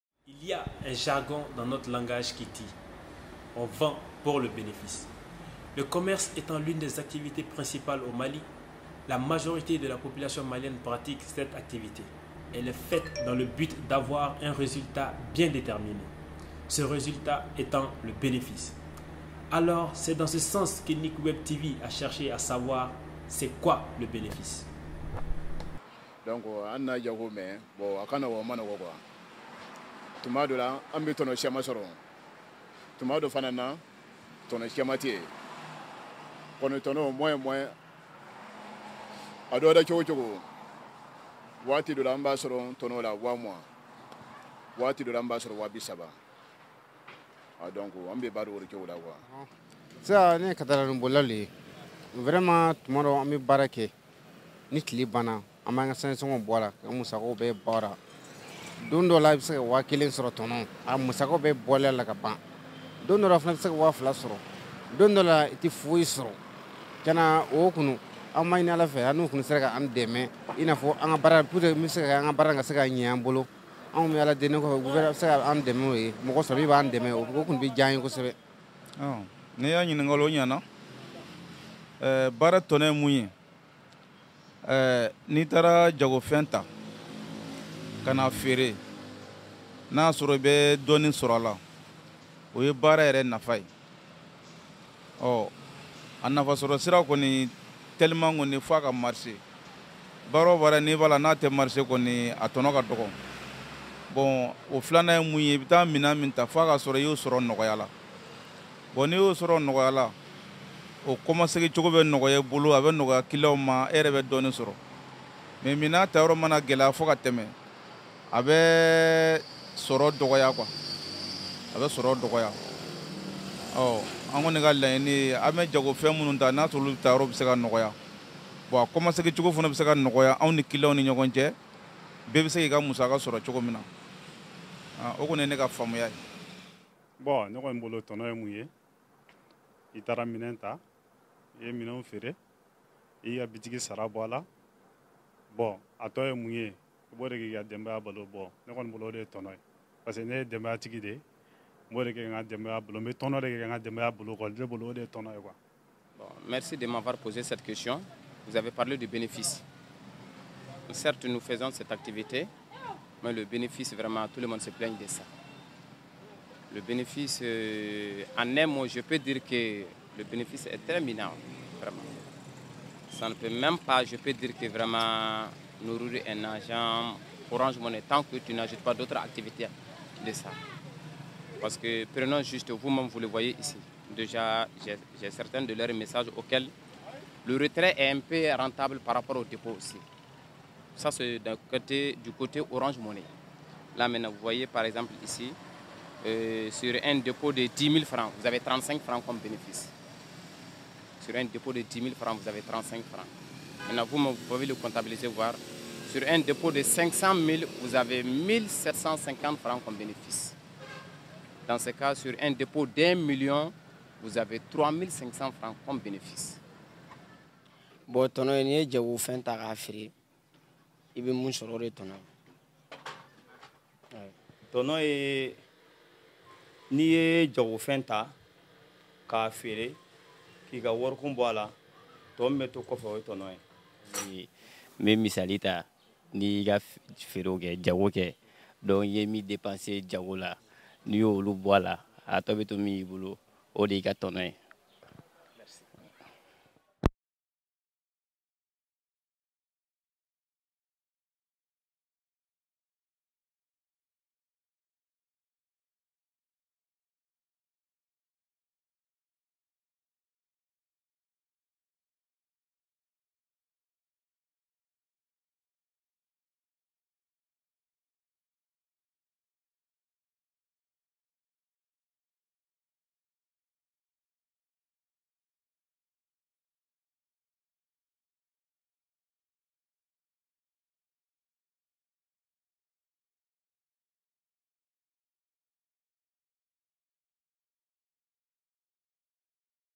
Micro Trottoir : Le bénéfice dans le commerce quotidien au Mali
Le bénéfice est un résultat comptable positif dégagé par une entreprise sur un exercice de 12 mois. Lorsque la différence entre les produits et les charges d'une société est positive, son résultat net affiche un bénéfice. Quand cette différence est négative, on parle de déficit ou de perte.